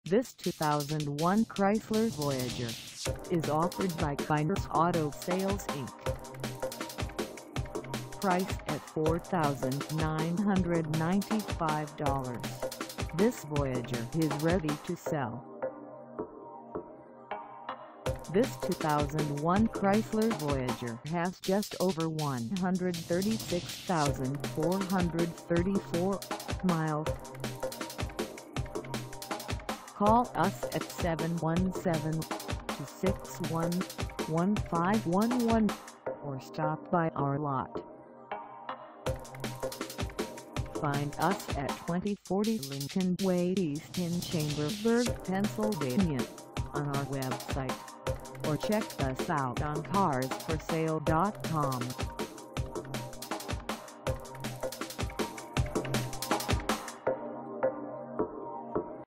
2001 Chrysler Voyager Base 4dr Minivan for sale in Chambersb
This 2001 Chrysler Voyager Base 4dr Minivan is for sale in Chambersburg, PA 17202 at Kyners Auto Sales INC.
Engine: V6 3.3L V6
Transmission: Automatic 4-Speed
Color: Green
Mileage: 136434
Contact Kyners Auto Sales INC 2040 Lincoln Way E Chambersburg, PA 17202 to test drive this 2001 Chrysler Voyager Base 4dr Minivan today.